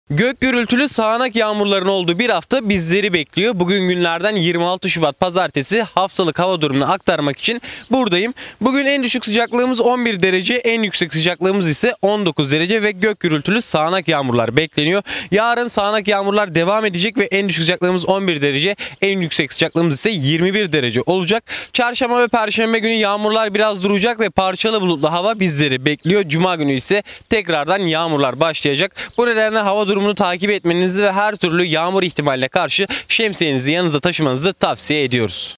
HAVA DURUMU
GÖK GÜRÜLTÜLÜ SAĞNAK YAĞIŞLARIN OLDUĞU BİR HAFTA BİZLERİ BEKLİYOR!

26 ŞUBAT - 3 MART HAFTALIK HAVA DURUMU